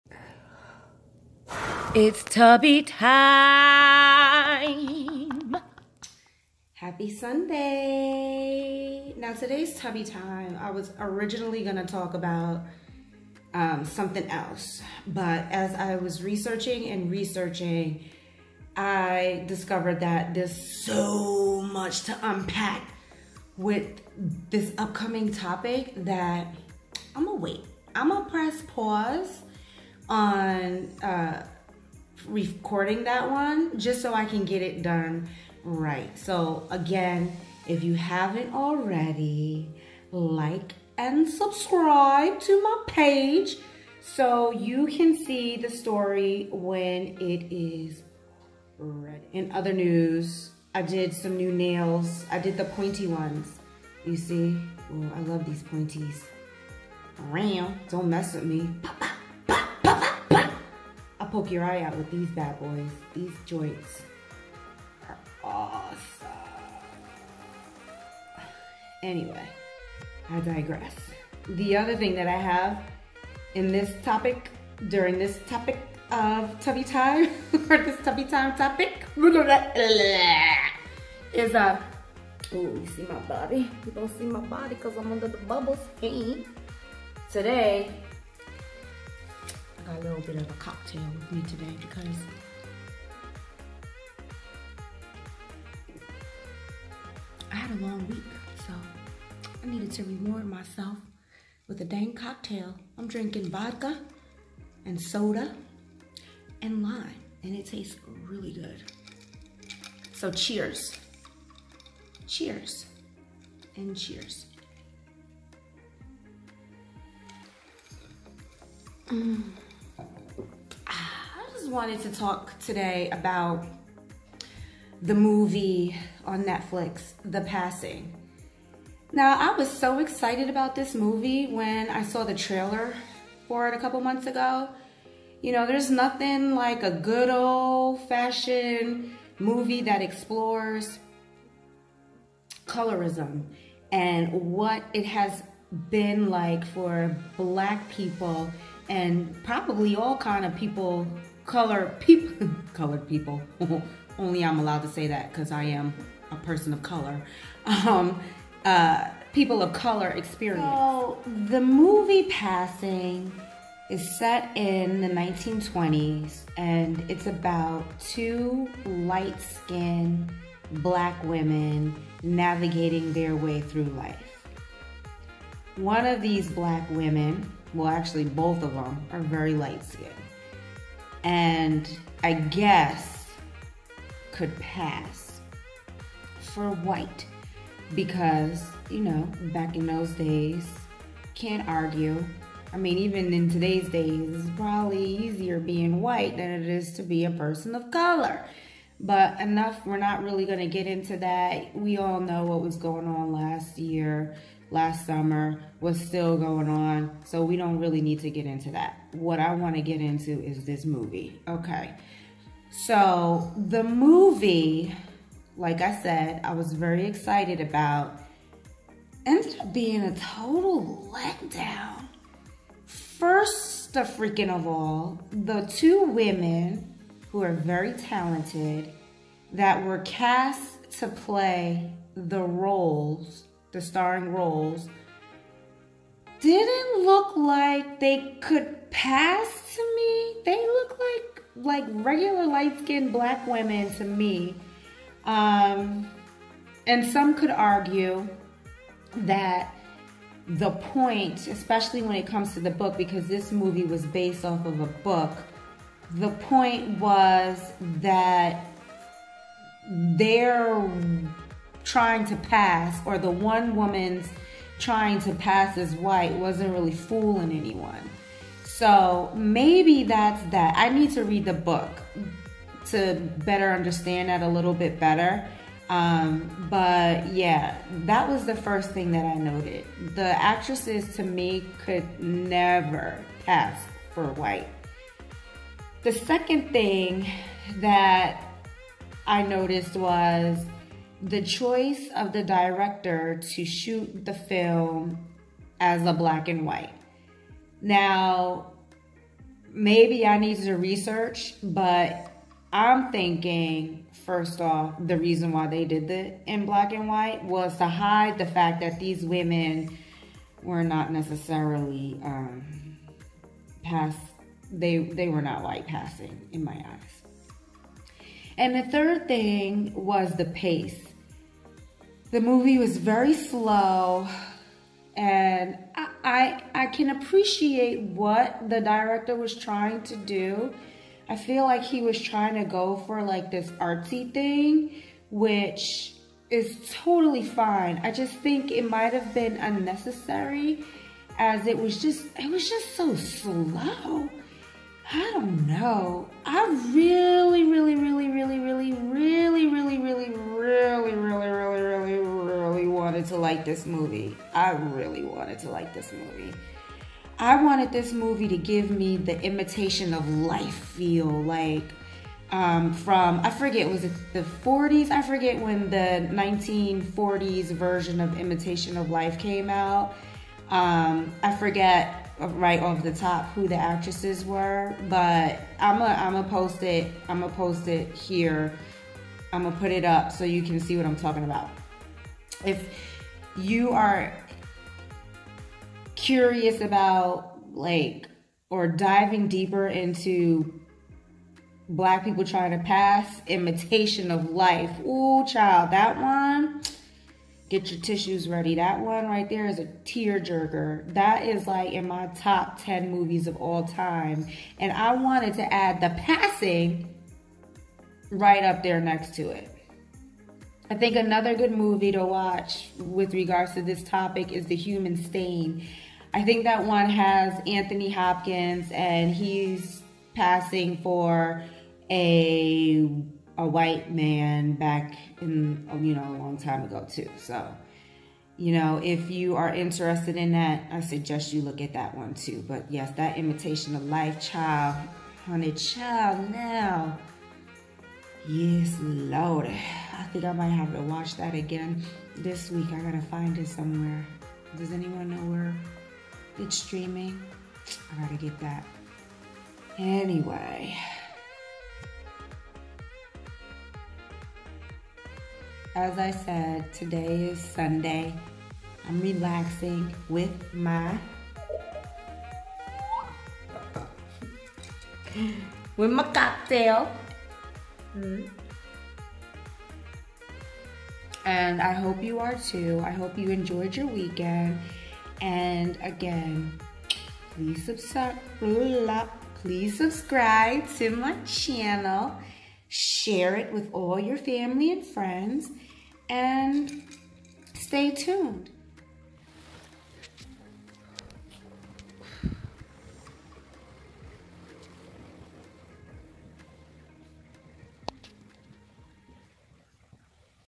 Tubby Time: Passing, A Movie Review
A quick review of the Netflix movie Passing